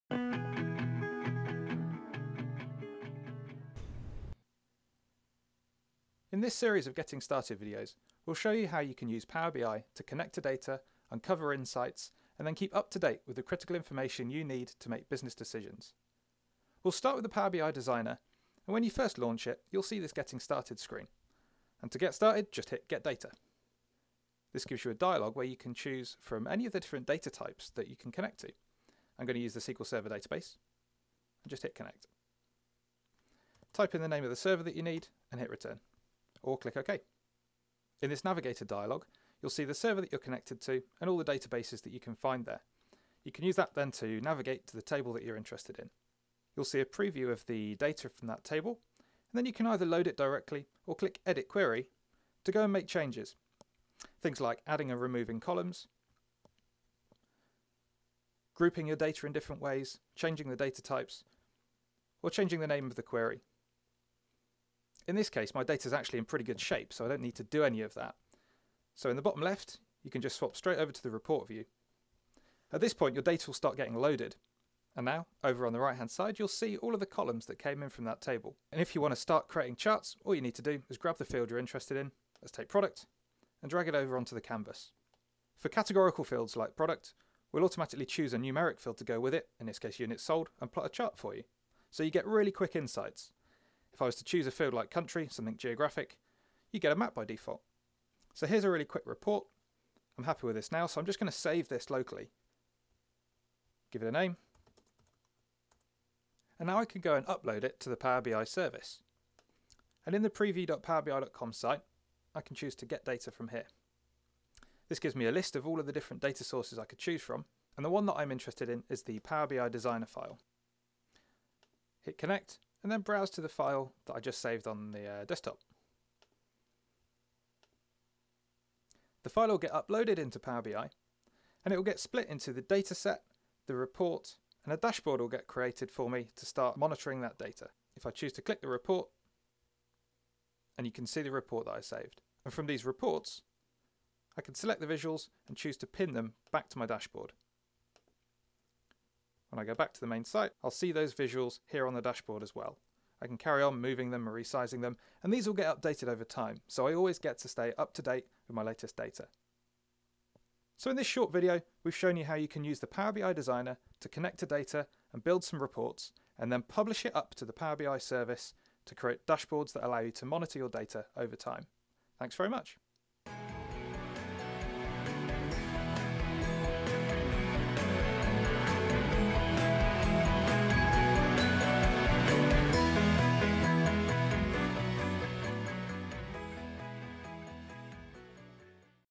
Getting Started with the Power BI Designer
The Power BI Designer Preview allows you to build interactive reports to use with dashboards on the Power BI Preview service. Get Started now!

Sign up for the Power BI Preview